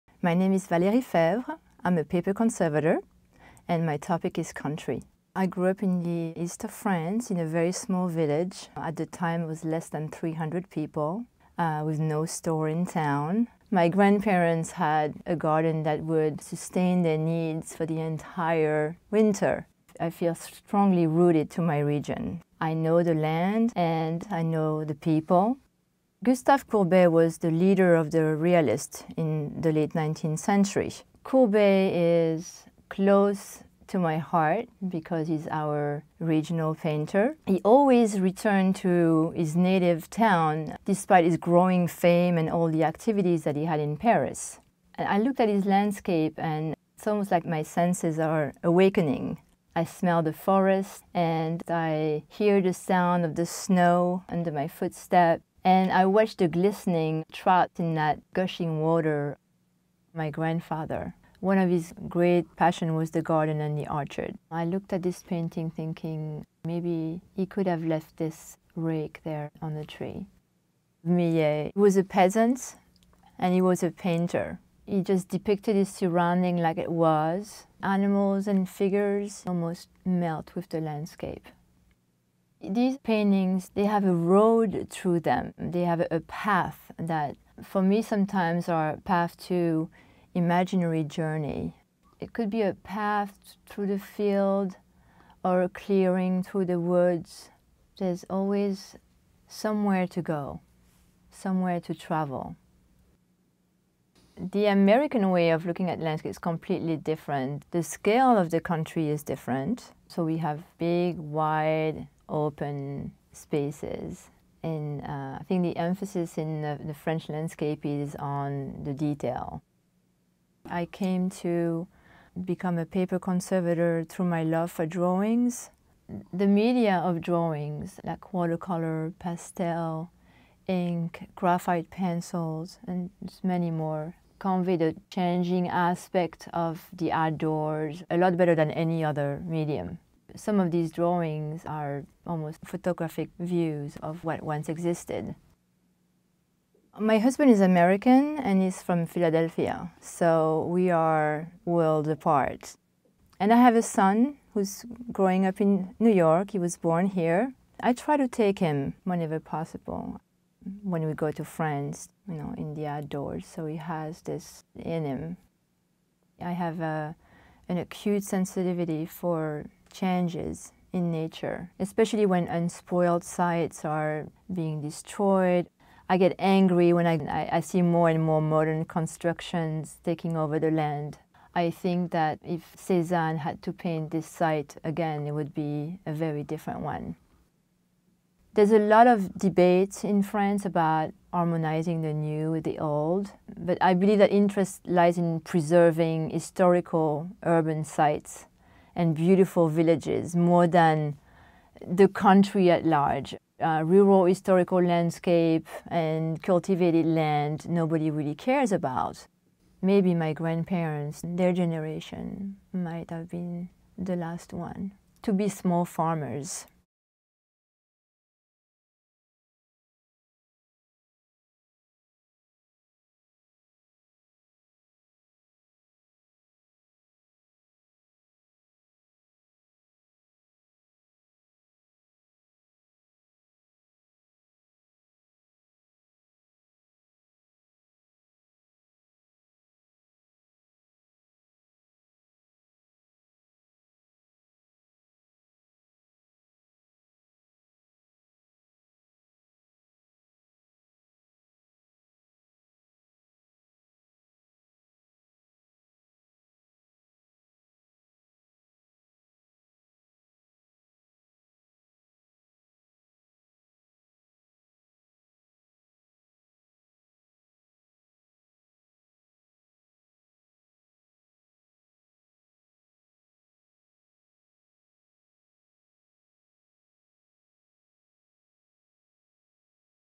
Connections: Country
"I feel strongly rooted to my region. I know the land and I know the people."

Paper conservator Valerie Faivre brings a French perspective on country scenes, scenes which are disappearing as more and more land is developed.